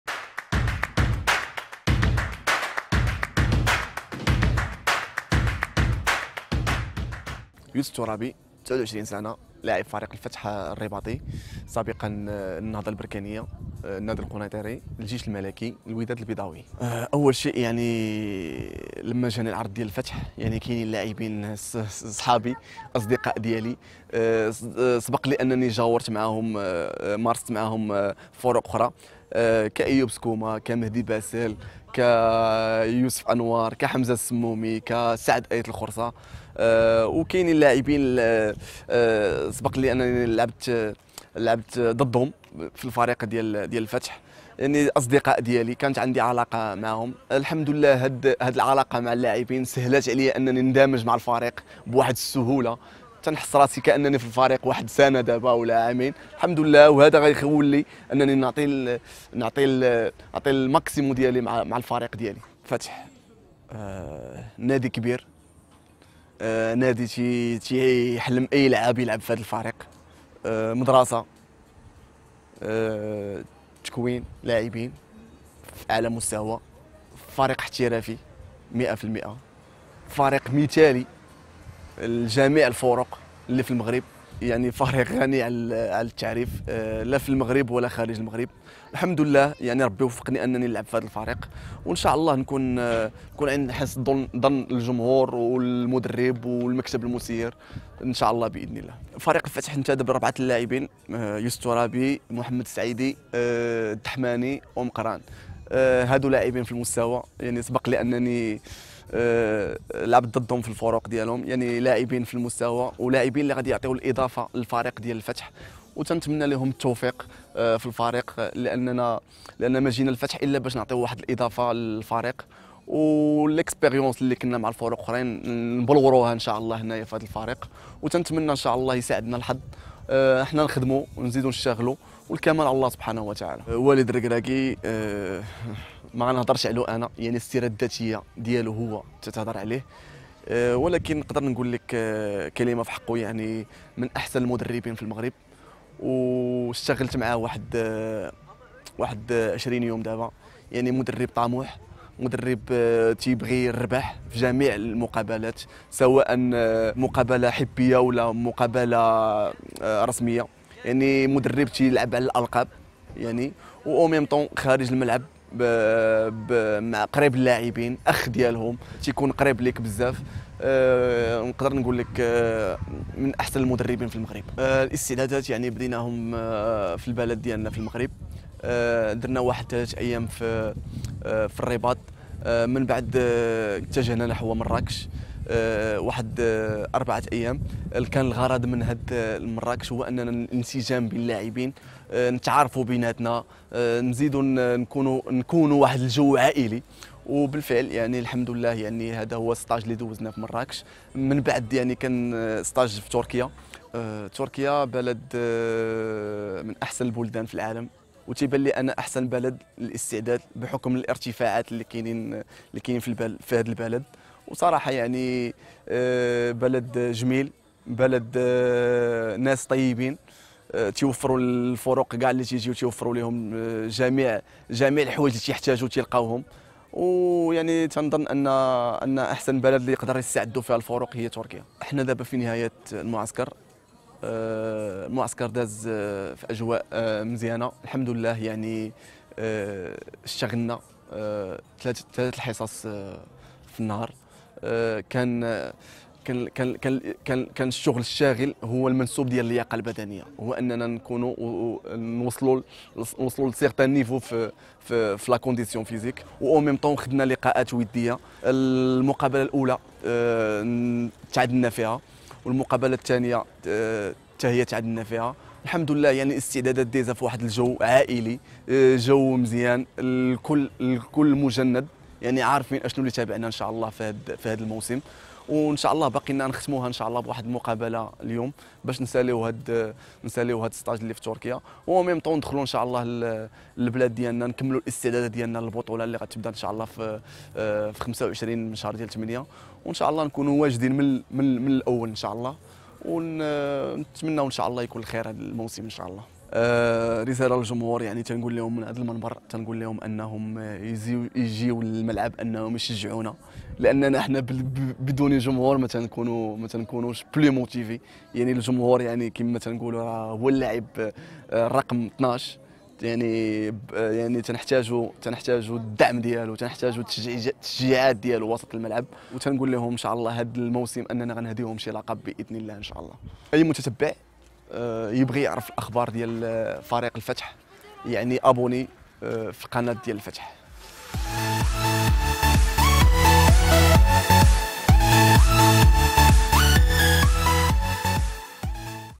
YOUSSEF ETTOURABI: "UN CLUB MODÈLE" / يوسف الترابي :" نادي نموذجي"
Découvrez en exclusivité la première interview du nouveau fussiste Youssef Ettourabi depuis son arrivée au sein du club de la capitale. Un entretien lors duquel le nouveau défenseur du FUS revient sur son choix de rejoindre les rangs du club.

تابعوا أول تصريح حصري للوافد الجديد على نادي الفتح الرياضي، يوسف الترابي، يتحدث فيه عن أسباب اختياره حمل ألوان نادي الفتح الرياضي في الموسم الحالي.